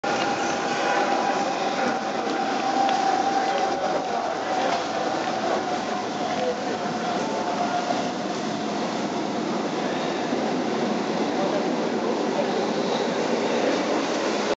Nuevo esquema para la linea B
Nueva coloracion para los coches mitsubishi de la linea B del subte de Buenos Aires. Facilmente se aprecia que en realidad se trata de un ploteo. Esto nace a partir el mal estado en que queda la pintura luego de que en un esfuerzo para sacar definitivamente los grafittis (roñosos por cierto) del subte.